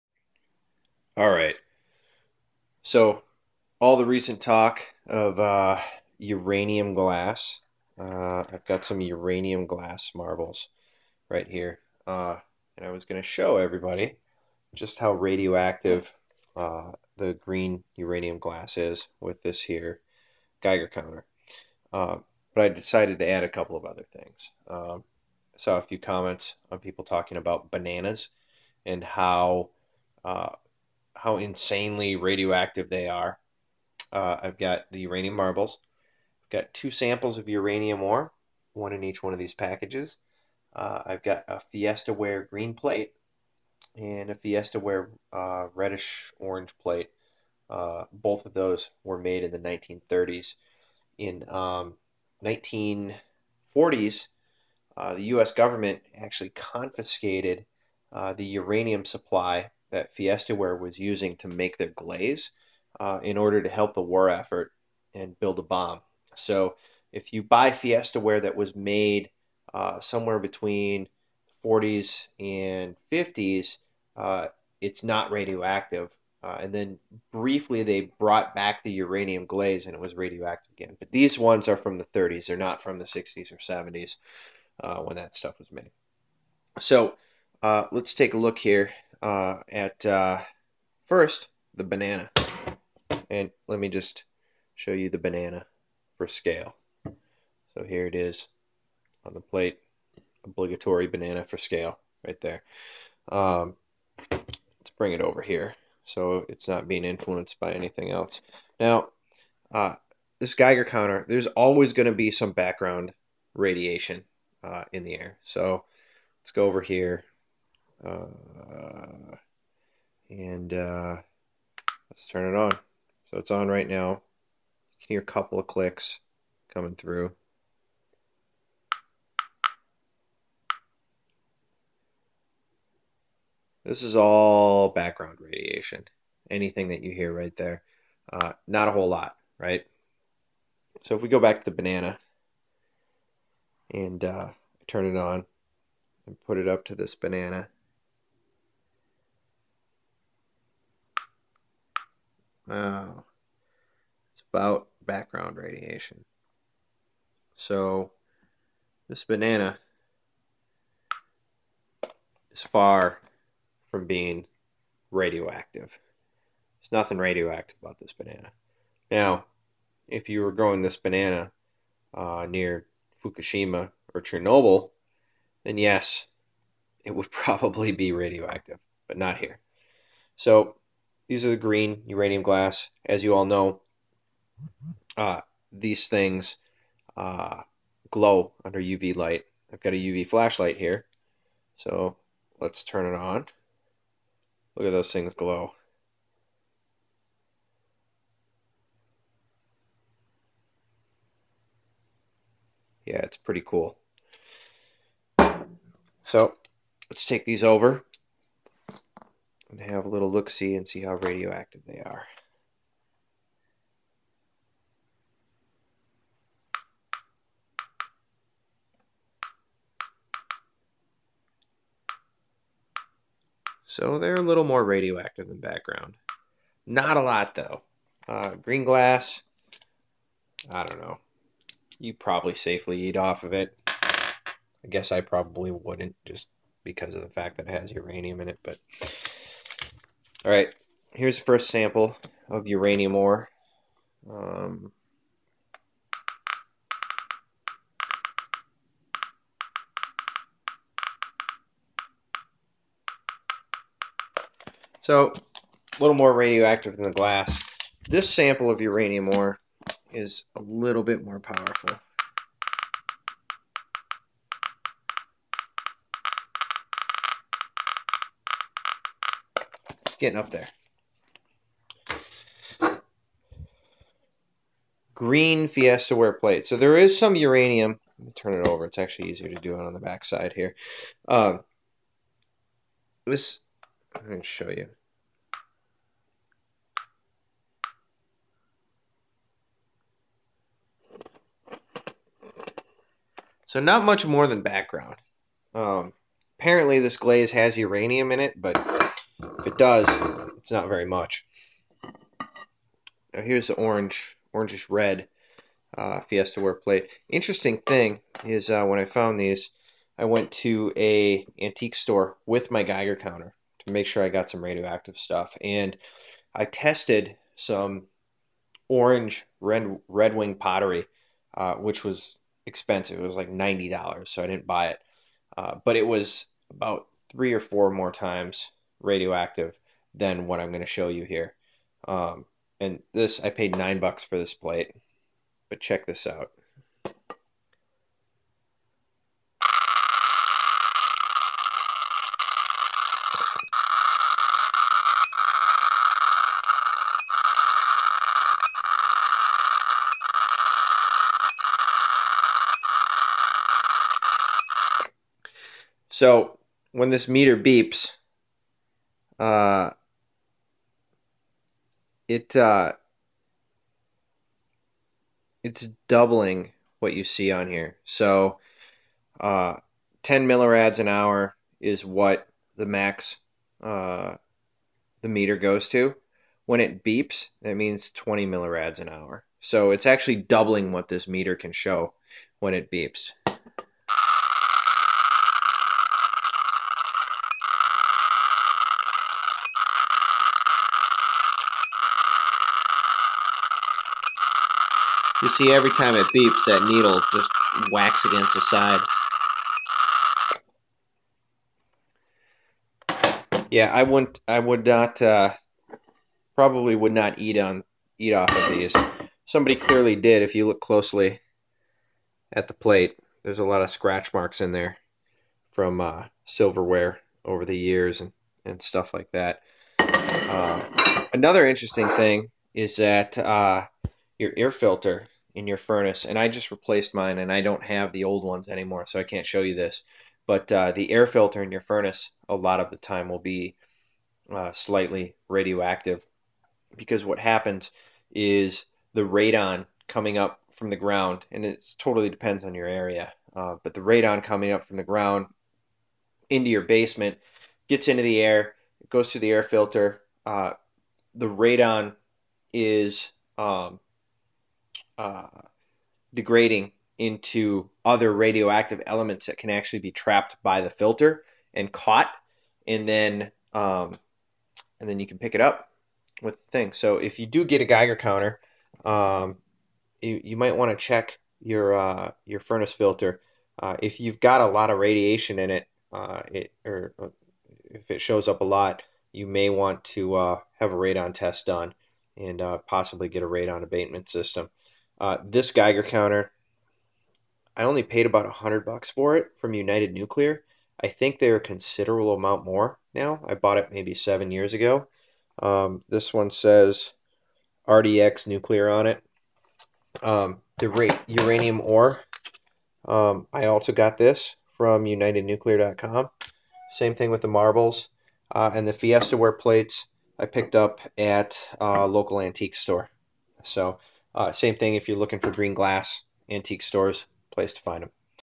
Green Uranium Glass an other things measure with Geiger counter
I measured a banana, green uranium glass, uranium ore, and Fiestaware plates with a Geiger counter.  Fun.

 If you want to buy this stuff, I recommend these items:

Geiger Counter

Uranium Ore